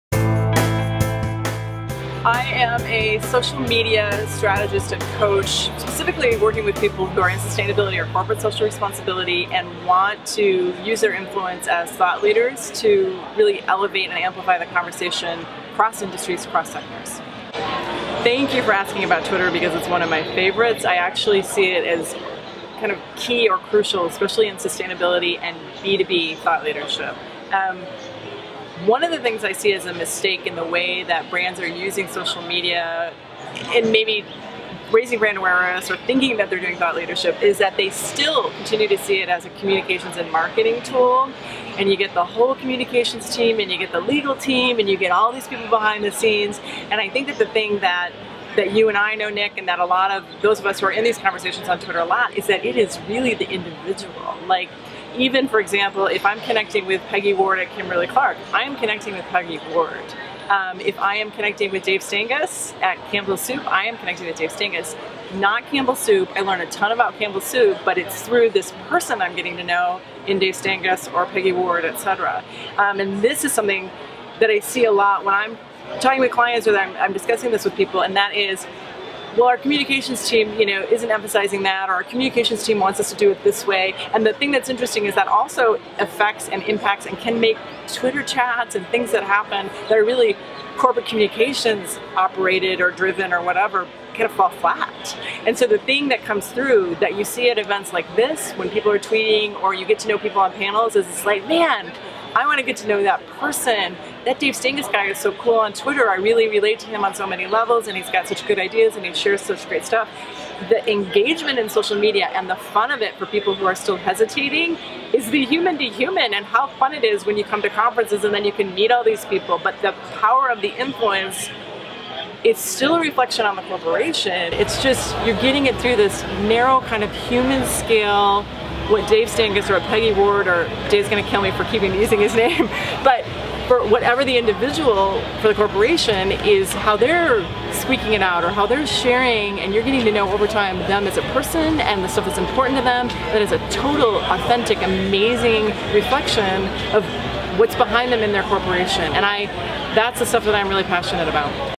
Andrea Learned Talks About Twitter at SXSWeco
At SXSWeco Andrea Learned talks about the importance of the individual in social media and how successful companies need to stop thinking about twitter as a communications and marketing tool.  It's really about the individual behind the account.